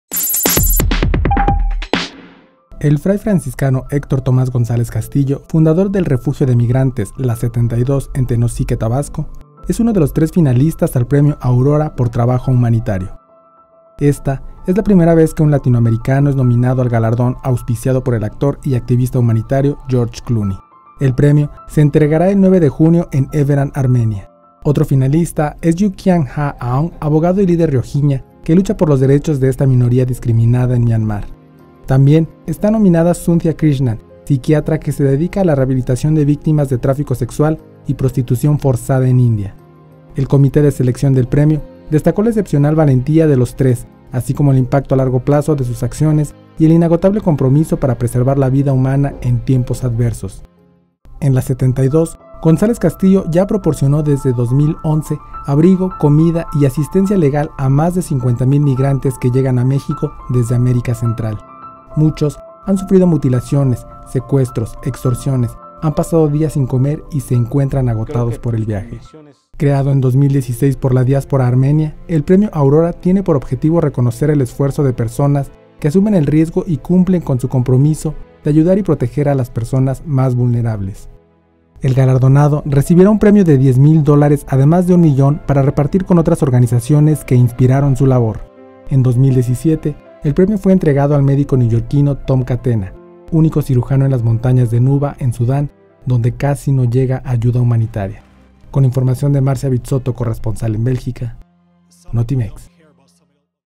Misionario mexicano es finalista a premio humanitario internacional
Esta es la primera vez que un latinoamericano es nominado al galardón, auspiciado por el actor y activista humanitario Geroge Clooney